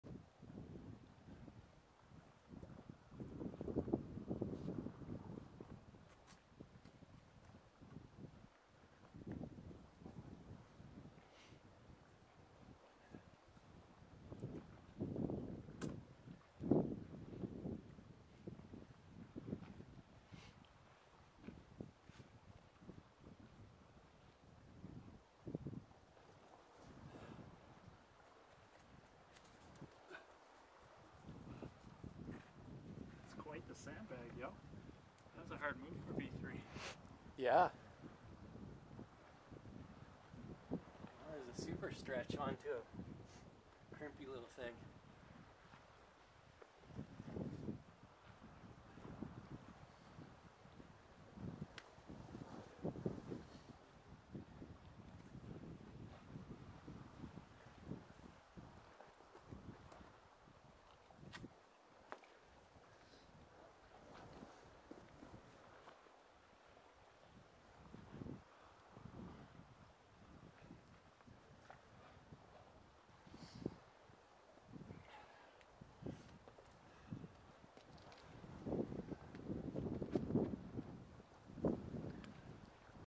Steve's Addiction @ Polly's Cove, Proper
Bouldering clip from the CNS Mobile Beta Project - aiming to provide video and GPS for all Nova Scotia classic boulder problems on your mobile phone.